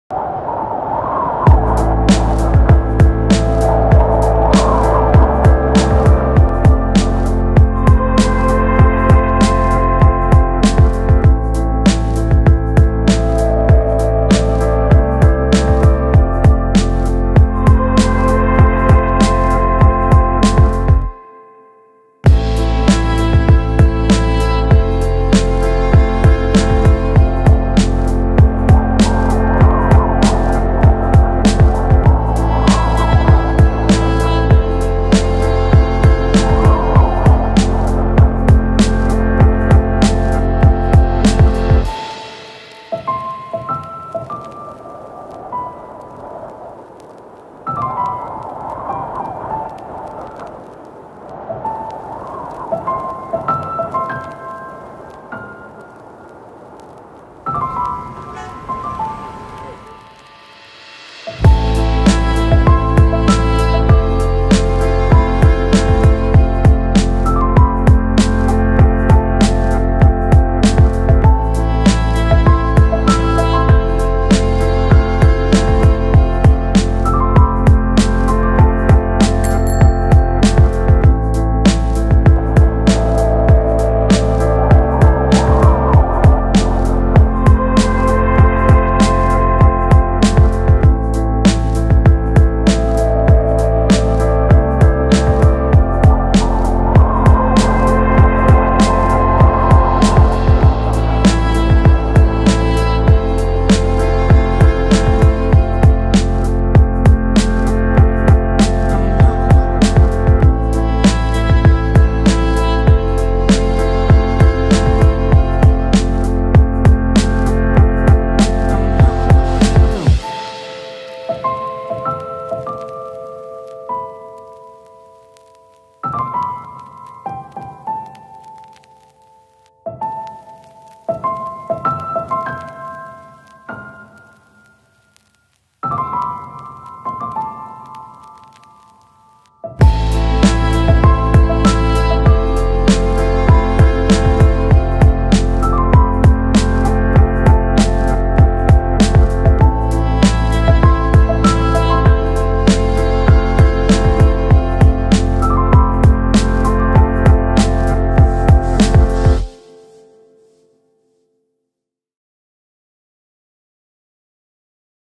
Amsterdam in Thick Fog
Amsterdam from above in Thick Fog. Timelapse & Hyperlapse video from canals, bikes, boats and a beautiful view over Amsterdam from different locations in the city. Some parts from this video are captured from the roof of the A`DAM Tower, just below the highest swing of Europe situated on the roof of the ADAM Loukout.

Camera Gear: Sony A7R III / Sony RX100 M3
Gimbal: Feiyu Tech a2000
Software: Adobe Premiere Pro